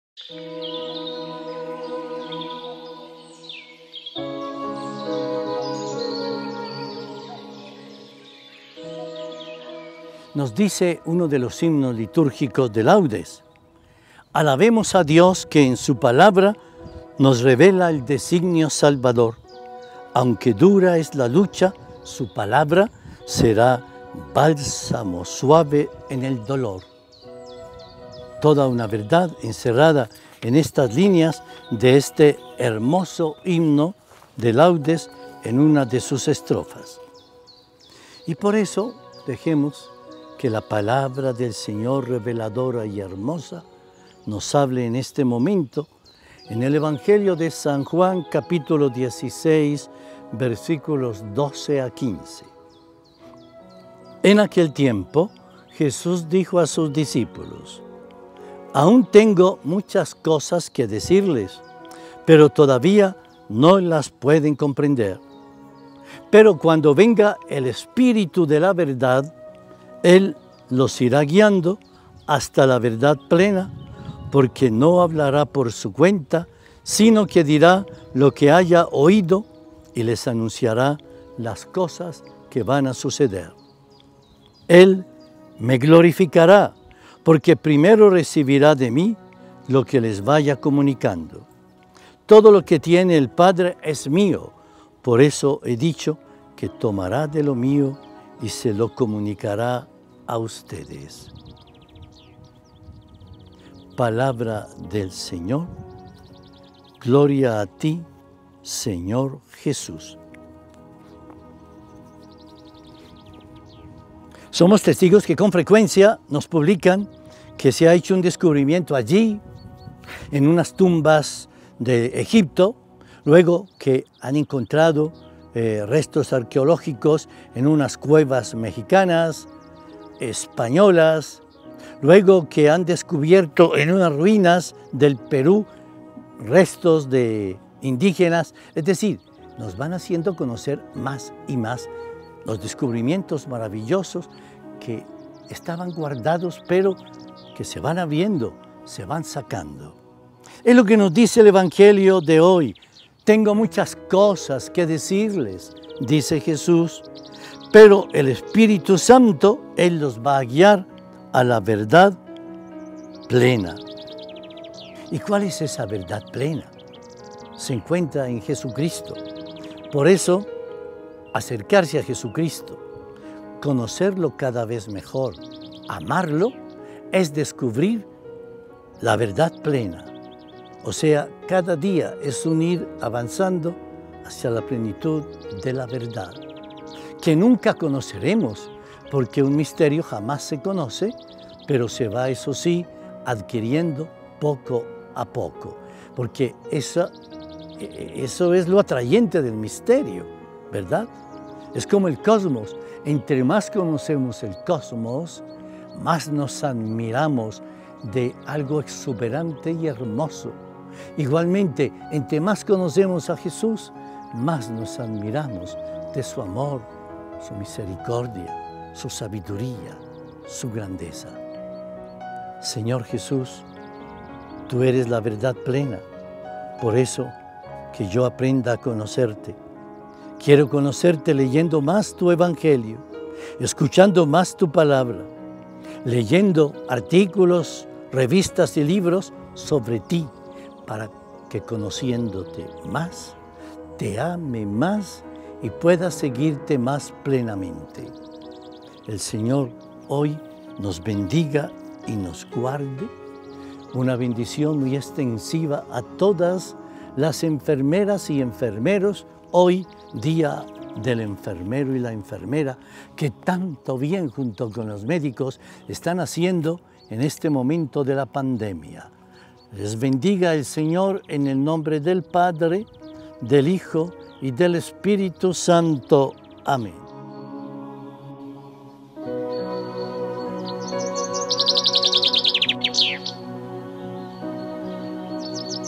Evangelio de hoy miércoles 12 de mayo de 2021, EL PAN DE CADA DÍA, Arquidiócesis de Manizales
Evangelio de hoy miércoles 12 de mayo de 2021 San Juan 16, 12-15

Lectura del santo evangelio según San Juan 16, 12-15:

En aquel tiempo, dijo Jesús a sus discípulos:
«Muchas cosas me quedan por deciros, pero no podéis cargar con ellas por ahora; cuando venga él, el Espíritu de la verdad, os guiará hasta la verdad plena. Pues no hablará por cuenta propia, sino que hablará de lo que oye y os comunicará lo que está por venir.
Él me glorificará, porque recibirá de lo mío y os lo anunciará.
Todo lo que tiene el Padre es mío. Por eso os he dicho que recibirá y tomará de lo mío y os lo anunciará».